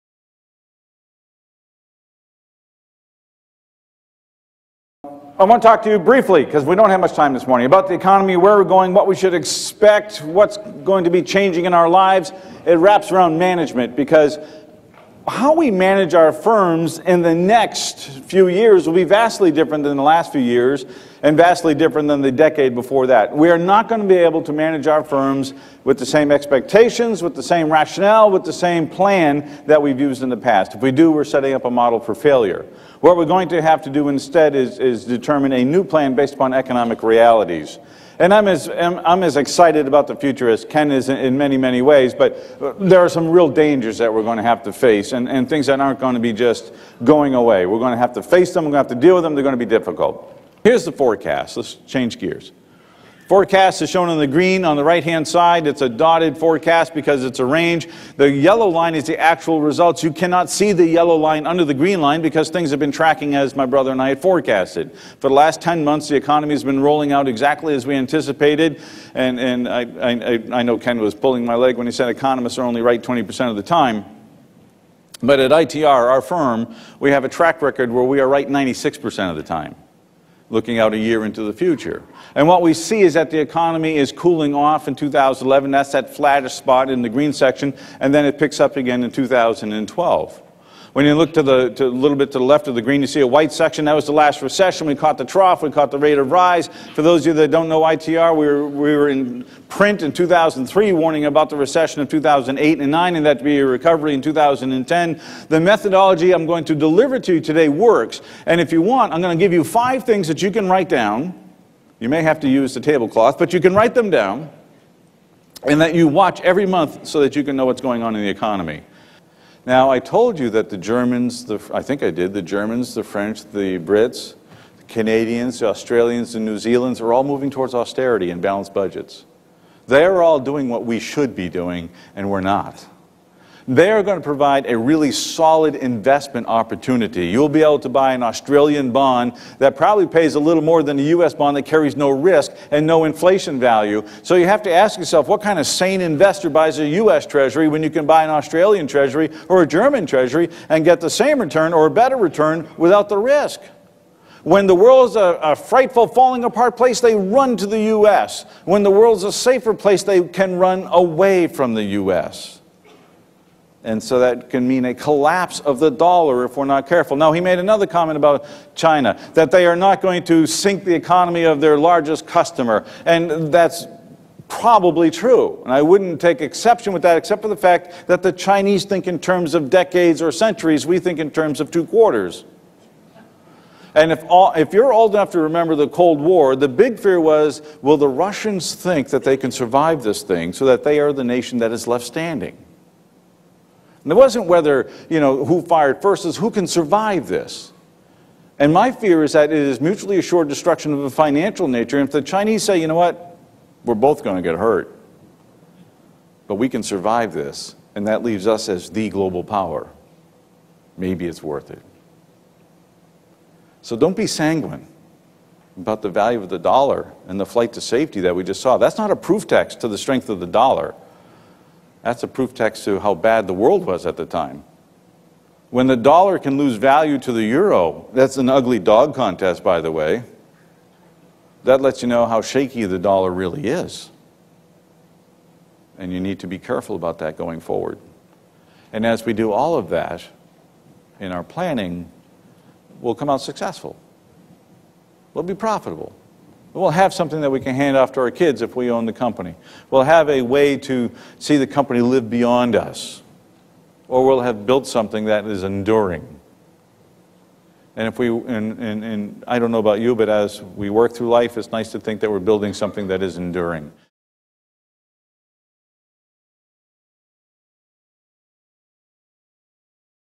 Alan Beaulieu: MFG Meeting
Alan Beaulieu, President and Principal of ITR discusses the US economy and economic trends.